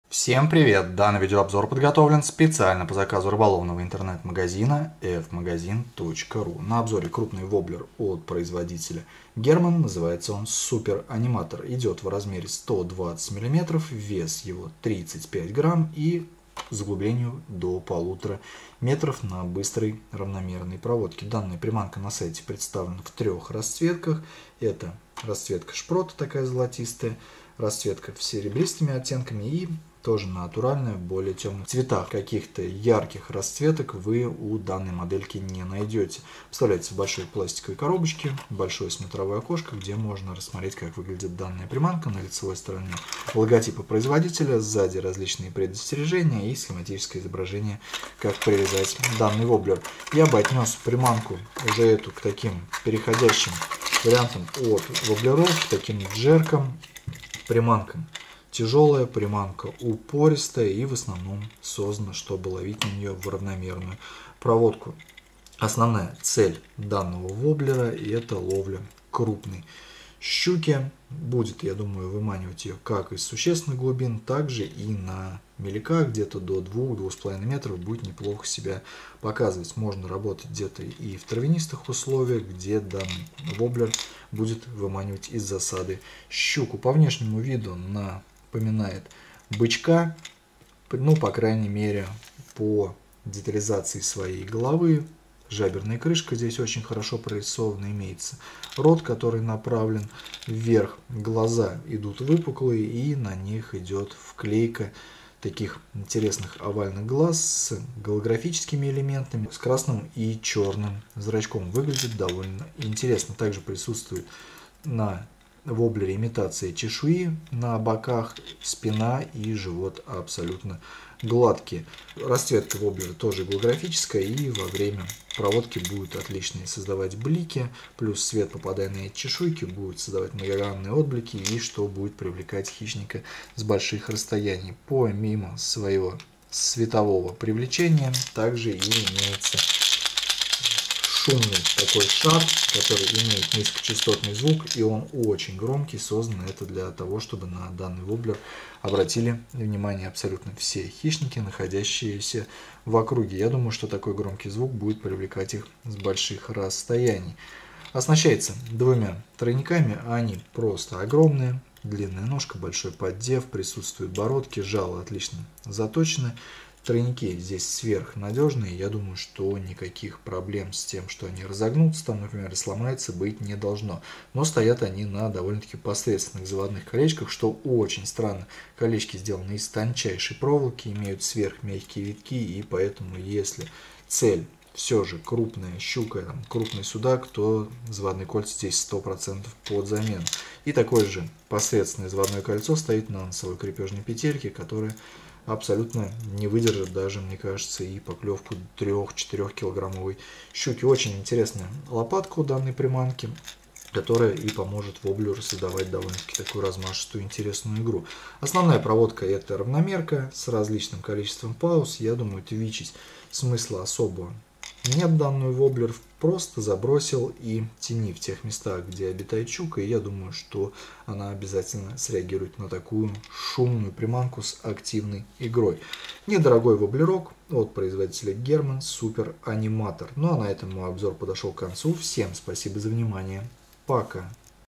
Обзор воблера German Super Animator по заказу Fmagazin
Воблер German Super Animator 120F крупный и тяжелый, для ловли за трофейным хищником на небольших глубинах.